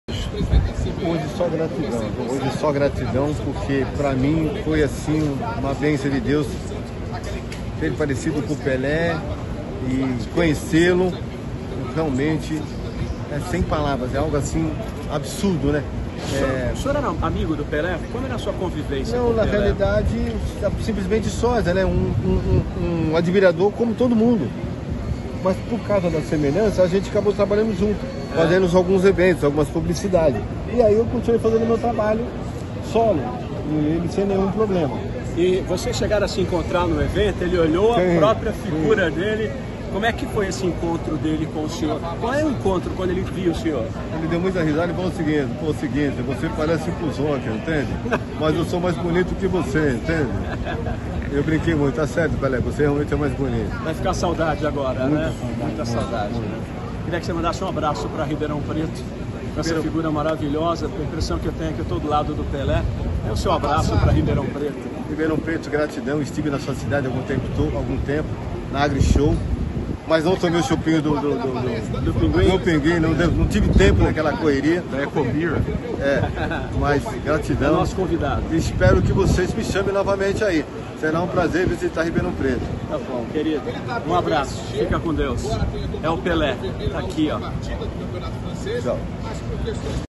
Sósia do Pelé
O SÓSIA MAIS PARECIDO COM PELÉ CHAMA ATENÇÃO E EMOCIONA PESSOAS DURANTE VELÓRIO DE PELÉ.